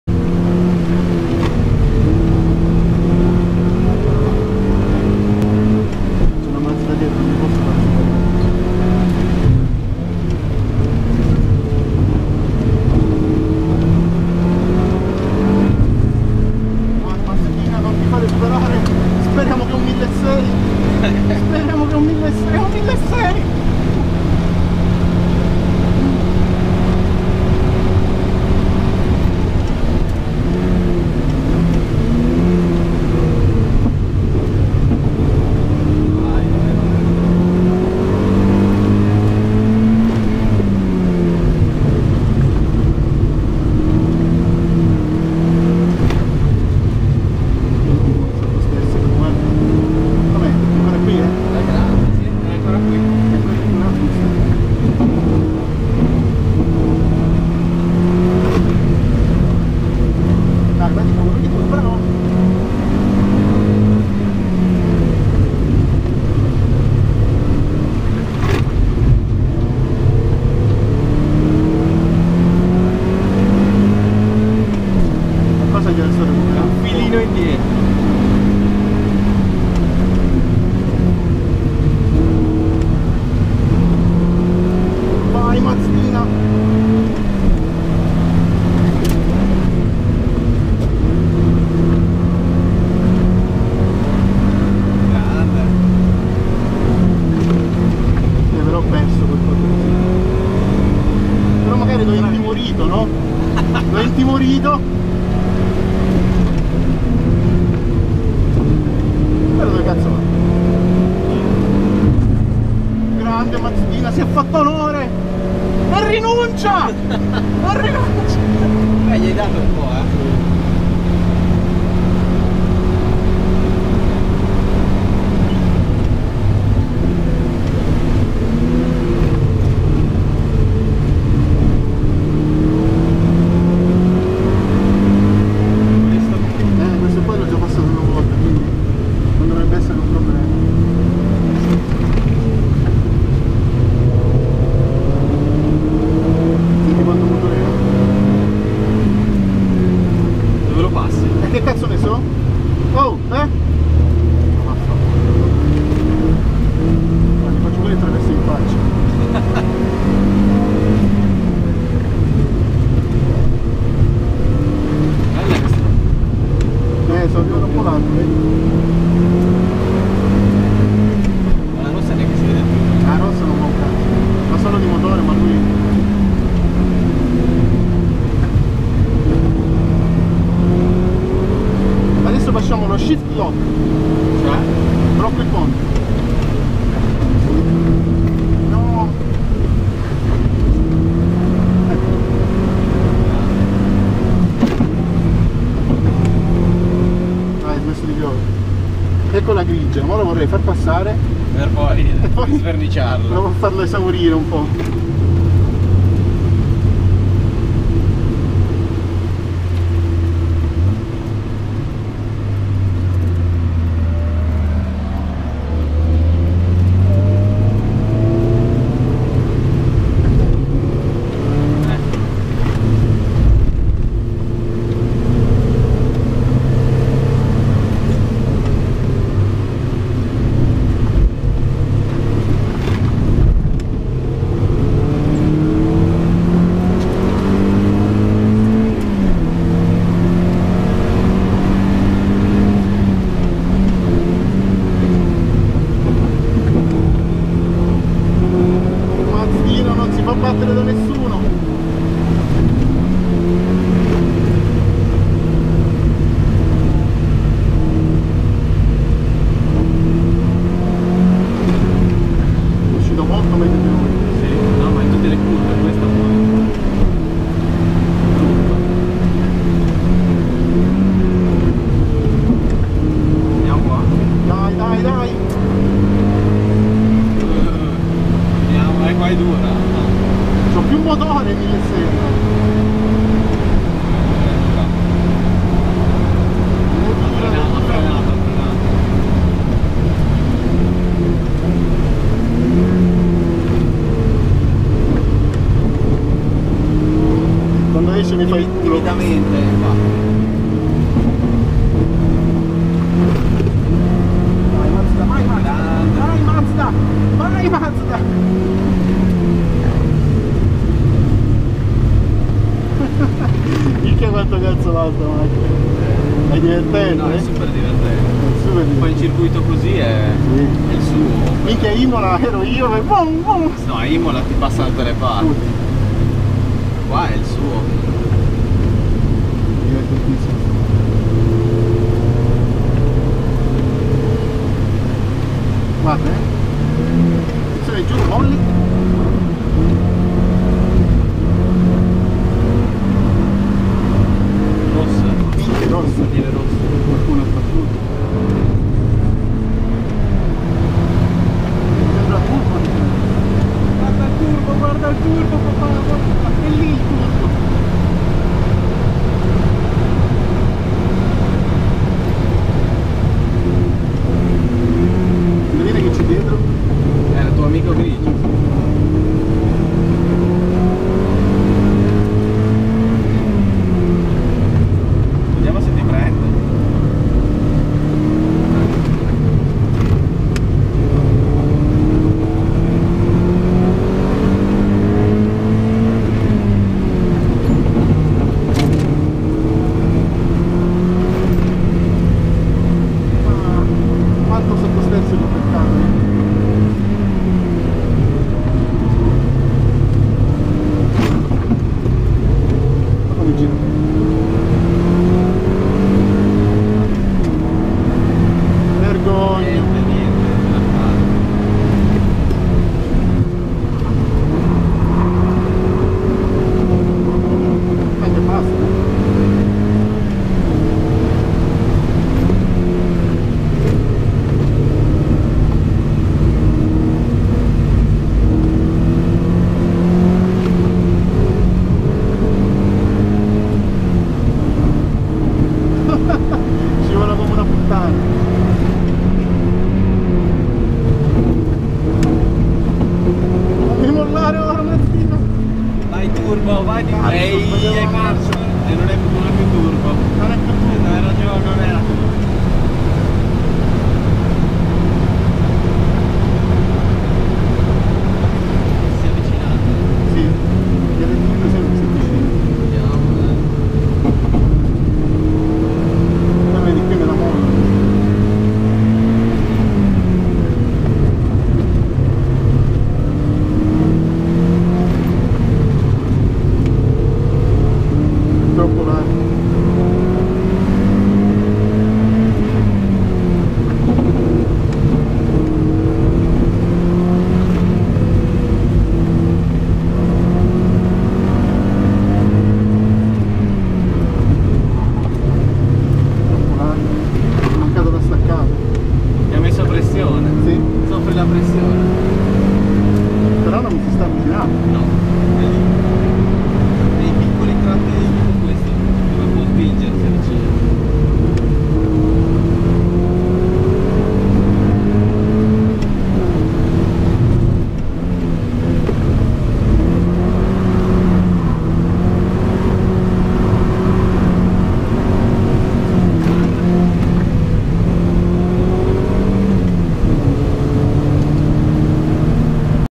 Mx5 1.8 vs Mx5 Japanese Car Meeting 29-05 RAIN DRIFT
Trolling a Mx5 and other cars during the last Japanese Car Meeting in Modena